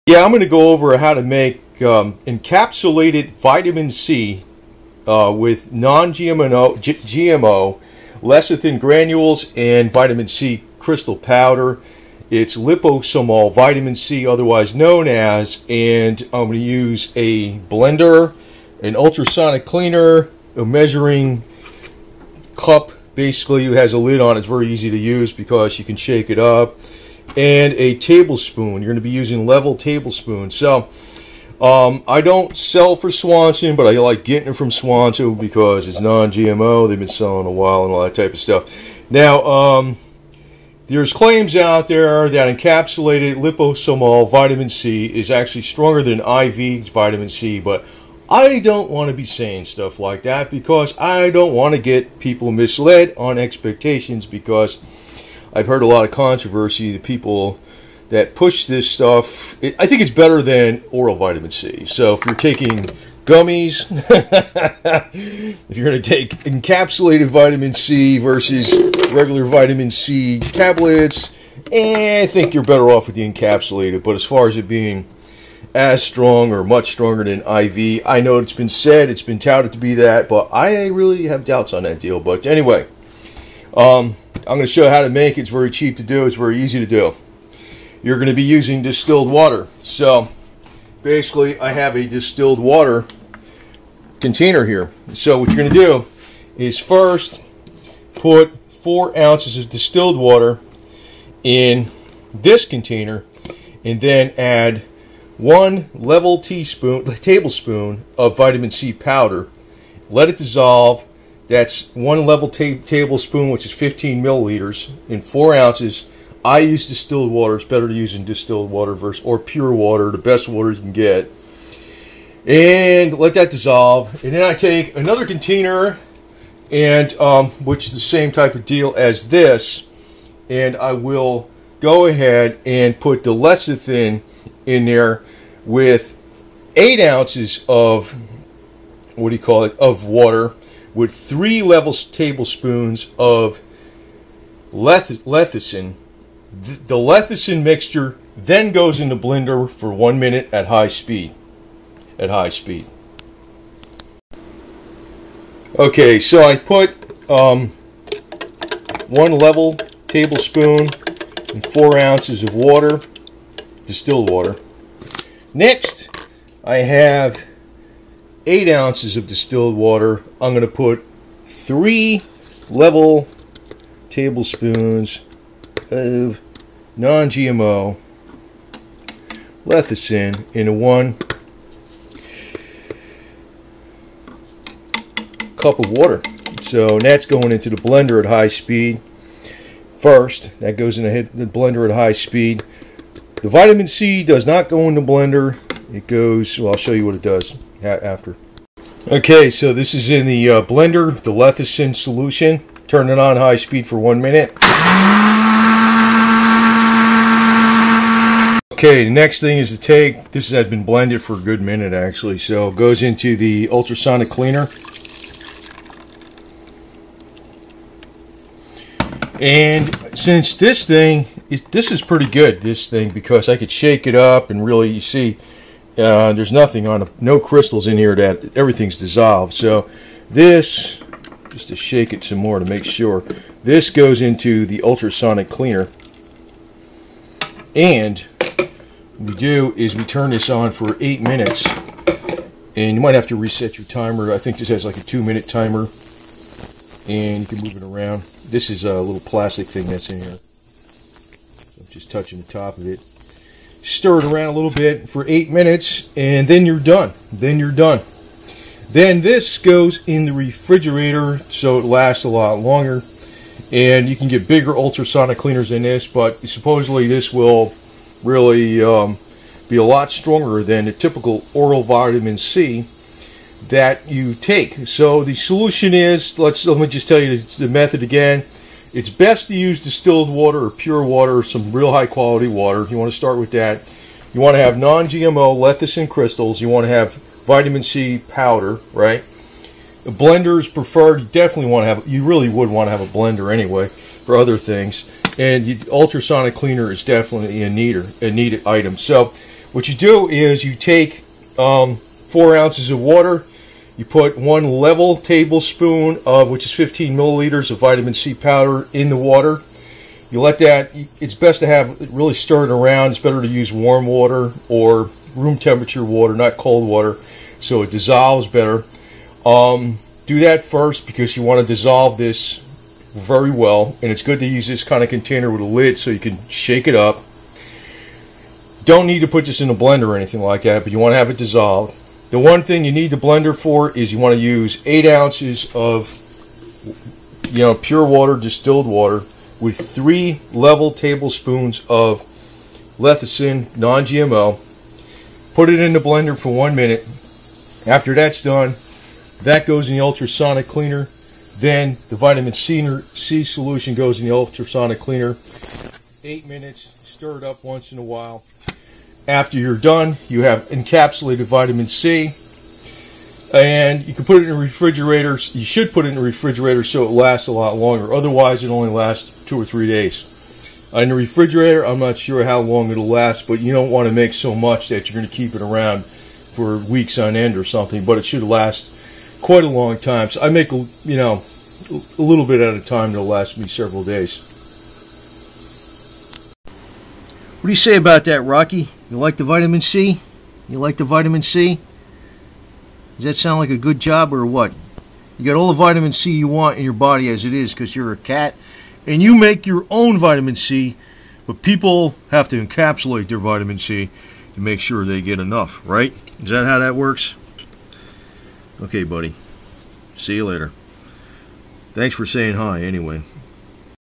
Encapsulated Liposomal Vitamin C -How to Make
Liposomal Vitamin C
Liposomal Vitamin C will enable far more of orally taken vitamin C to be absorbed into the body than standard vitamin C.  There are claims on the internet that liposomal vitamin C is even far more effective than intravenous vitamin C, but I have severe doubts it is that good, although it is still a more effective way to take oral vitamin C.

Items needed:
Vitamin C Crystals
Non-GMO Lecithin
Distilled Water
Ultrasonic Cleaner
Blender

1/2 cup warm distilled water
1 level tablespoon of Vitamin C

1 cup warm distilled water
Mix in 3 level tablespoons of
Non-GMO Lecithin

Put in the Blender
Blend at high speed for 1 minute

Put the mixed lecithin in an
Ultra-sonic clean

Then add the vitamin C mixture

Turn on the ultra-sonic cleaner for 8 minutes
Until the Vitamin C is encapsulated with the
Lecithin.

Refrigerate to make it last.